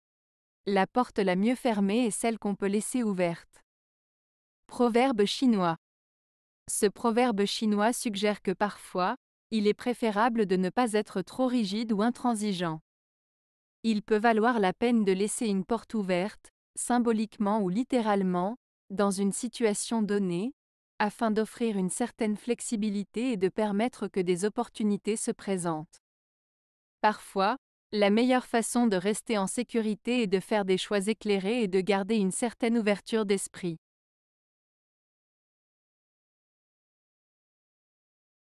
La porte la mieux fermée est celle qu'on peut laisser ouverte. - Proverbe Chinois
Ce proverbe chinois suggère que parfois, il est préférable de ne pas être trop rigide ou intransigeant. Il peut valoir la peine de laisser une porte ouverte, symboliquement ou littéralement, dans une situation donnée, afin d'offrir une certaine flexibilité et de permettre que des opportunités se présentent. Parfois, la meilleure façon de rester en sécurité et de faire des choix éclairés est de garder une certaine ouverture d'esprit.